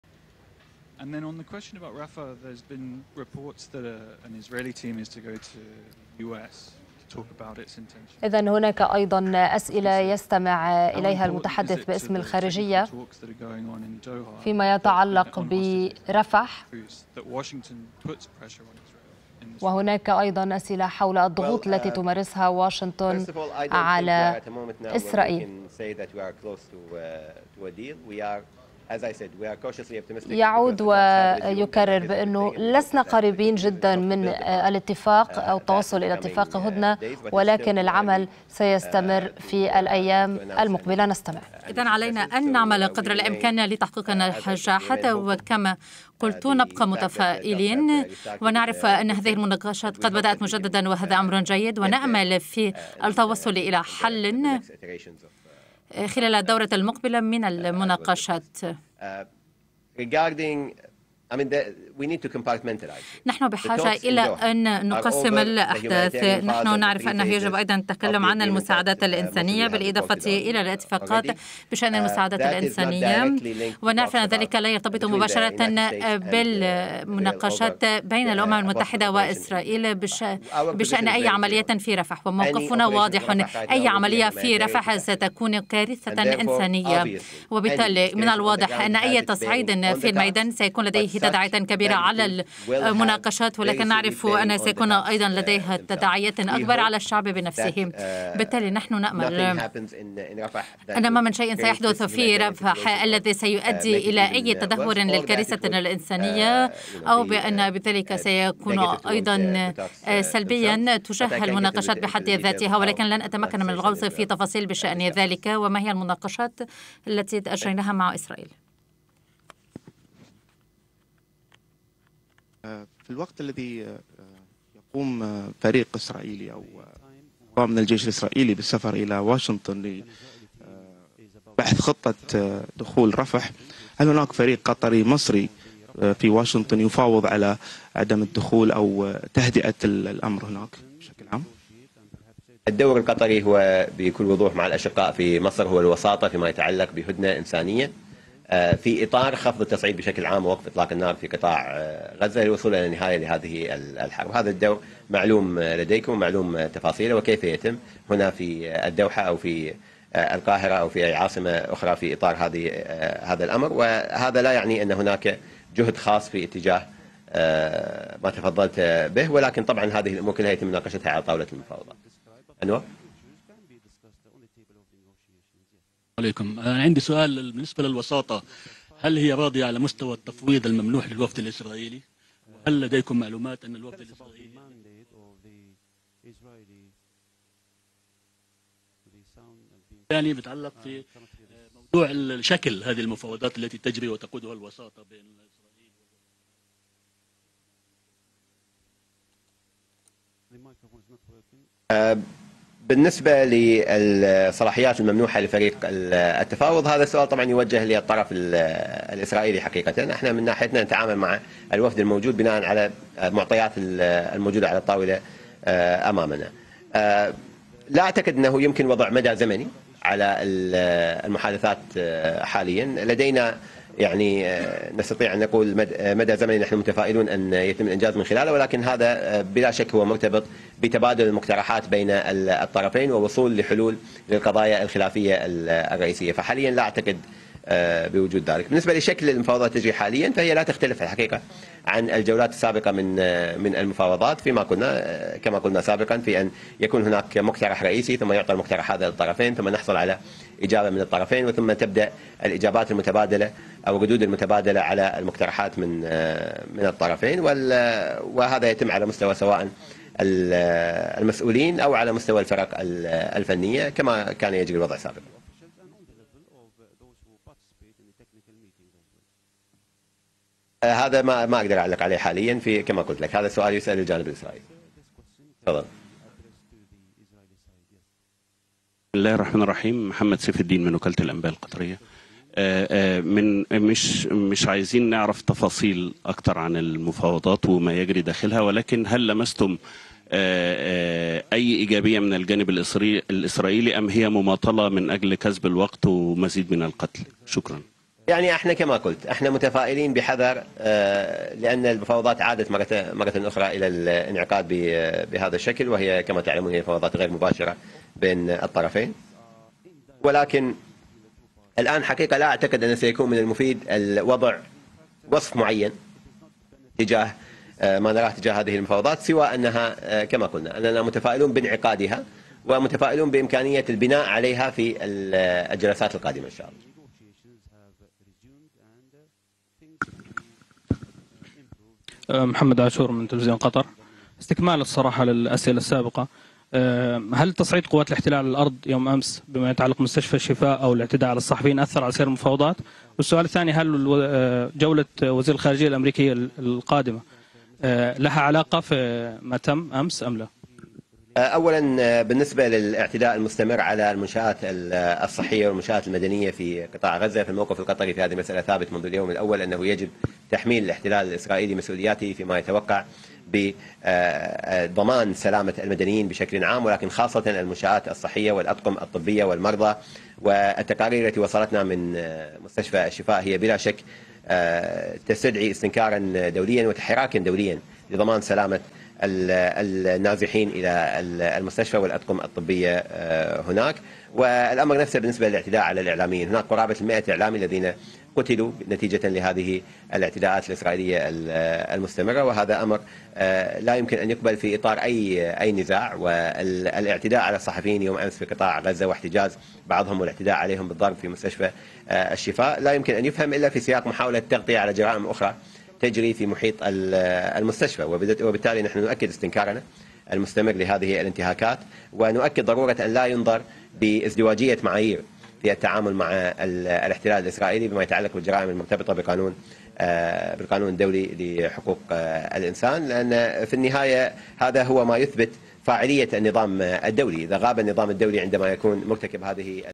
الخارجية القطرية: متفائلون بشأن مسار محادثات هدنة غزة والمفاوضات التقنية لا تزال جارية
الخارجية القطرية: الوقت لا يزال مبكرا للحديث عن أي اختراق أو نجاح في مفاوضات صفقة الرهائن والهدنة